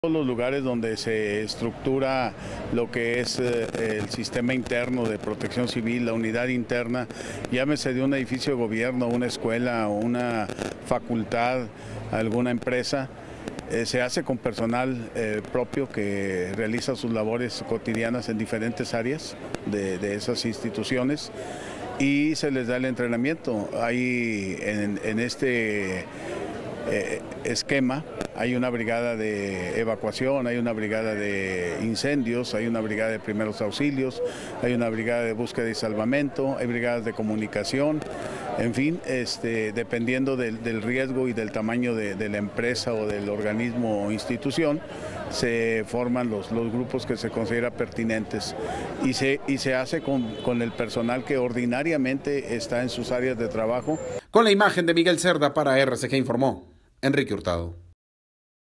Brigadas internas de protección civil
Después de informar que en el marco de la conmemoración del día nacional de protección civil, en saltillo se han llevado a cabo 600 simulacros en diferentes sedes, el titular de esta instancia, Sergio Robles, detalló la forma de operar las brigadas internadas de protección civil y cómo trabajan.